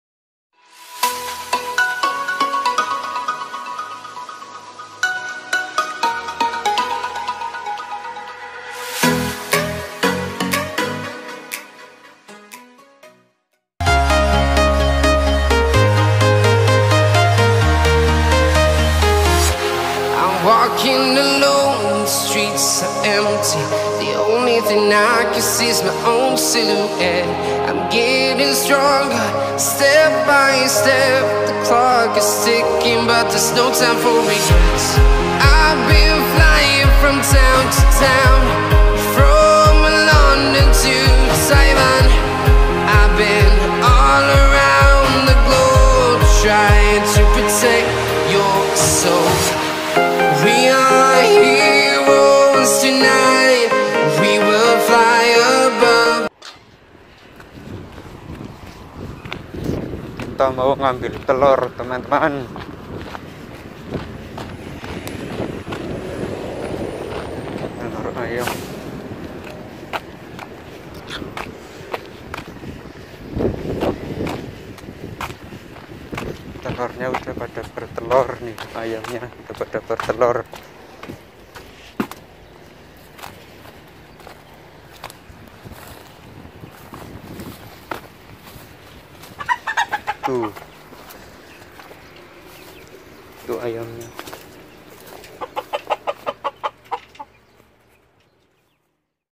DI KOREA KERJA BIKIN KANDANG SAPI,PULANG NYA DI KASIH TELOR AYAM .MANTAP
Kerja sampingan,bikin kandang sapi di Korea